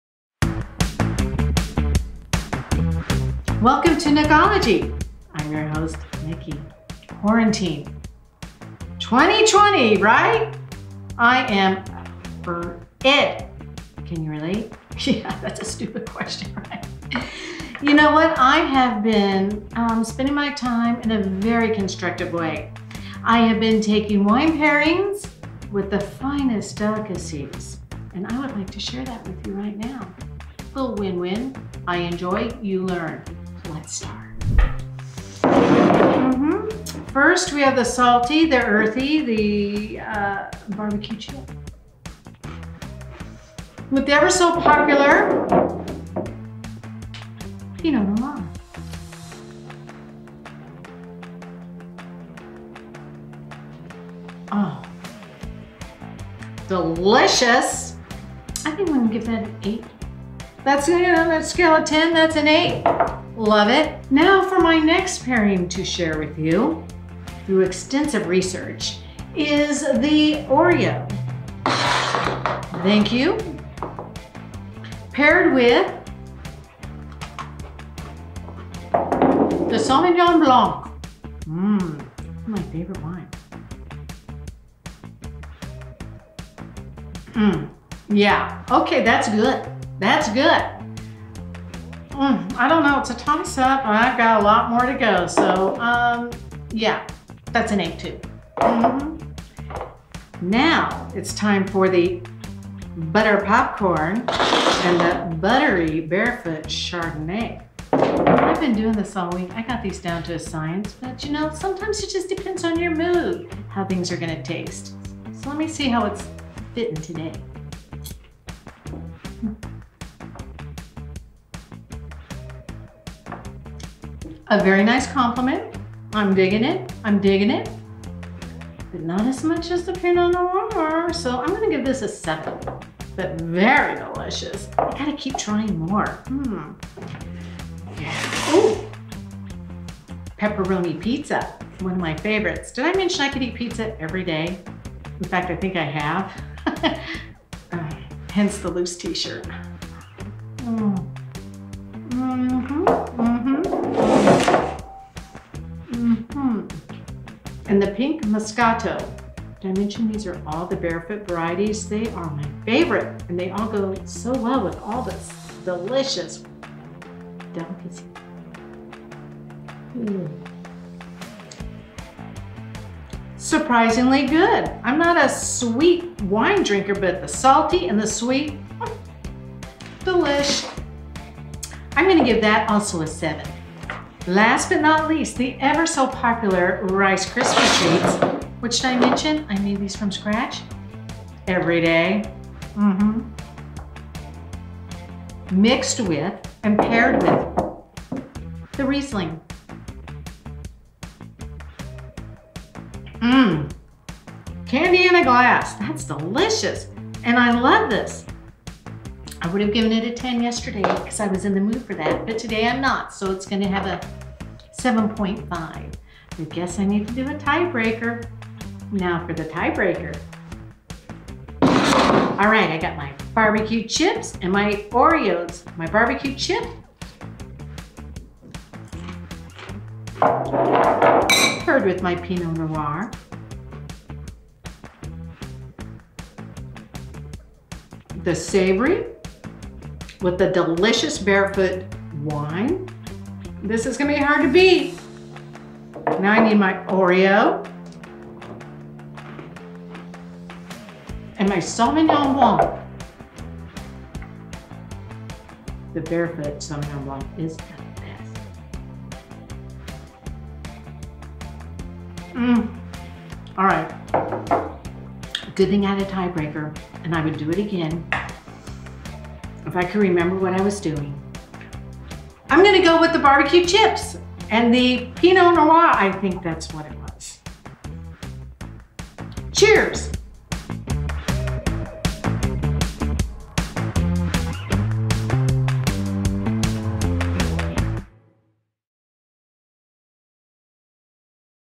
Nookology with Wine Nook--Quarantine Fun--Junk Food and Wine Pairings with Barefoot on Tap box wine
How do you pair junk food and wine? We tell you here! Barbecue chips and Pinot noir, oreos and sauvignon blanc, buttered popcorn and buttery barefoot chardonnay, pizza and Barefoot pink moscato, rice krispie treats and reisling